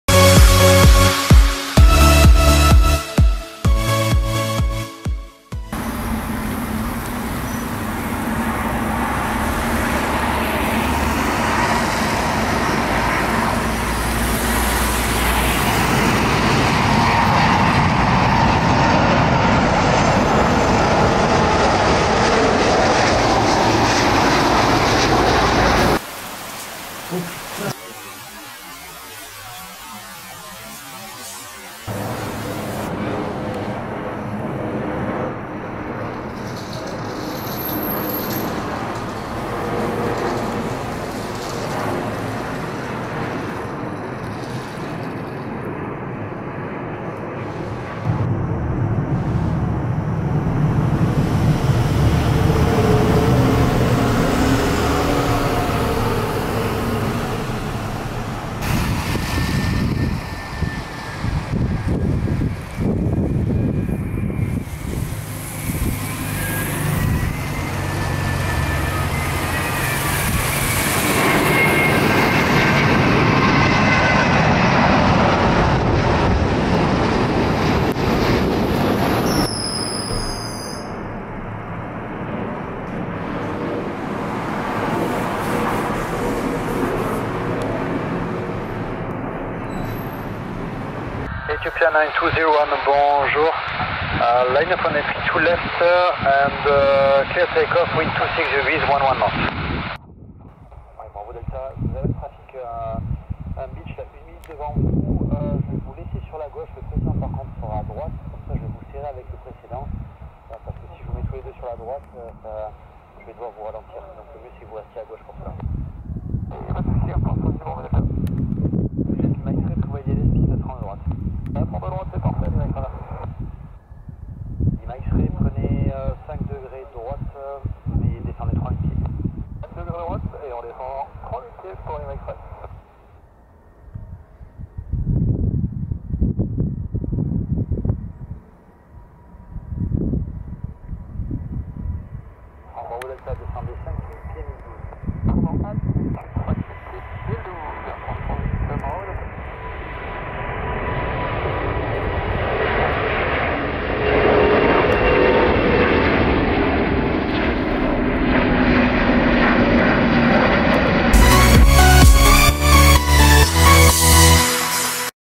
AIRBUS AIRCRAFT FACTORY | [A330 NEO - A350] - Night storm planespotting Toulouse-Blagnac LFBO !
November 2022

A330-200 MRTT
Landing
Check-Flight

A330-900 NEO DELTA AIRLINES
Landing
First Flight

First A330-800 NEO AIR GREENLAND
Go Around
Landing
Check-Flight

A350-900 XWB ETHIOPIAN AIRLINES
Delivery Flight
Ferry Flight
Takeoff

Night Storm Planespotting LFBO Toulouse-Blagnac
Full HD